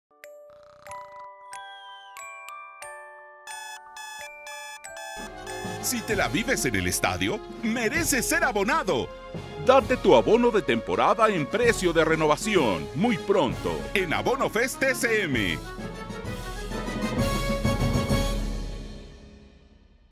Mereces ser abonado. Abonos 2019-2020
¿Te la vives en el estadio? 🙋‍♀️🙋‍♂️
¡Mereces ser abonado! 🇳🇬

Date tu abono desde $2,690 pesos*.
Próximamente en AbonoFest.

*Precio de renovación de ABONADO GUERRERO de Temporada 19-20.